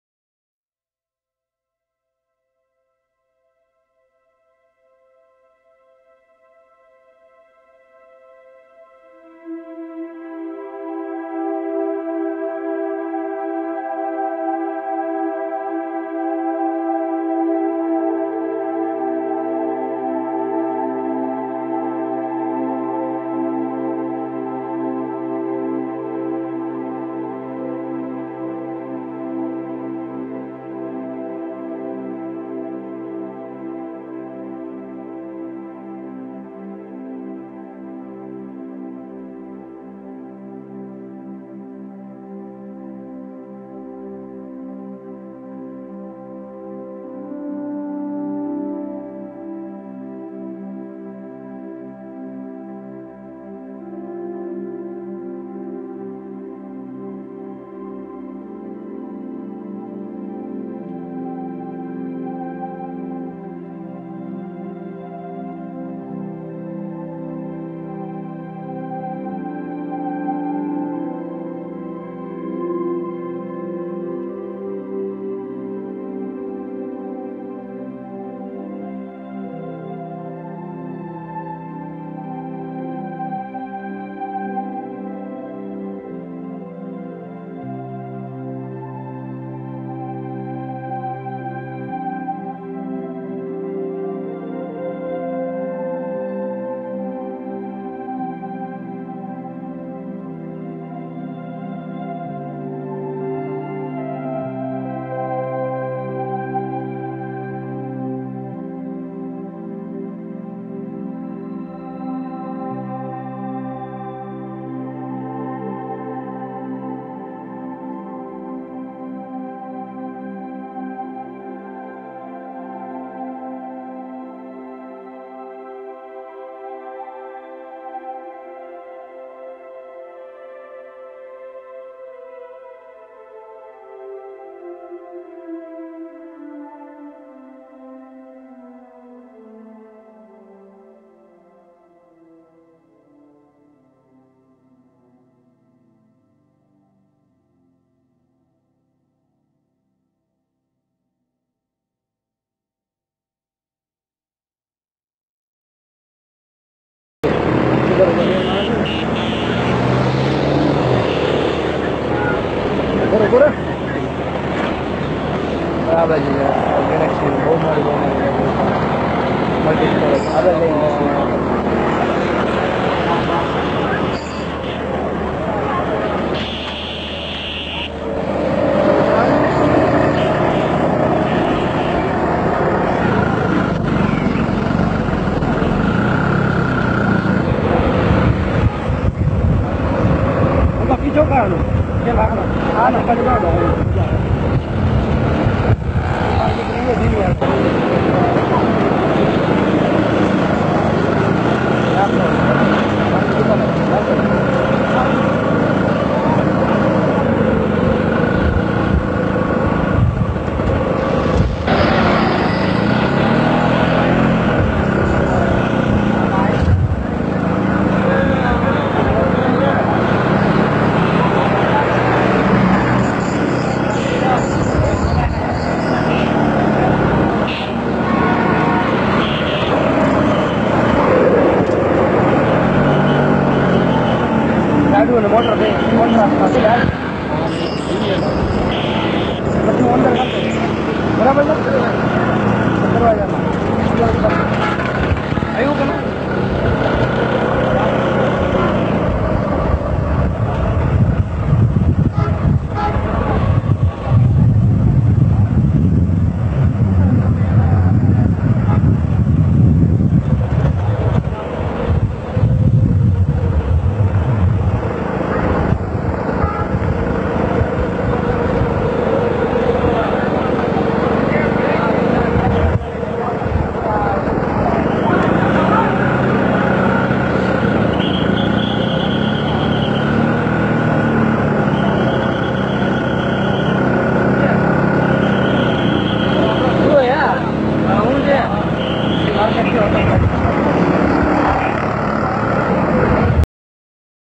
Old Video of Rajkot City- Must Watch
Are you a Rajkotian ? Enjoy the view of Rajkot City almost before 35 years (approx of year 1985, not exact idea).